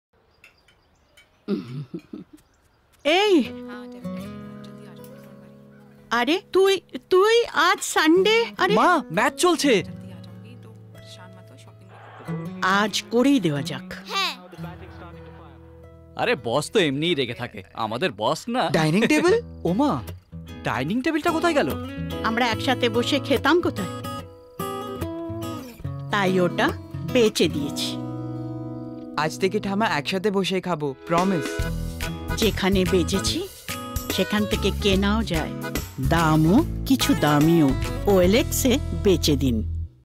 Dining table ki asli keemat (BEN)
Watch how Dadi gives a smart lesson on the fast eroding joys of family life. Keemat bhi Kuch Keemti Bhi sirf OLX par.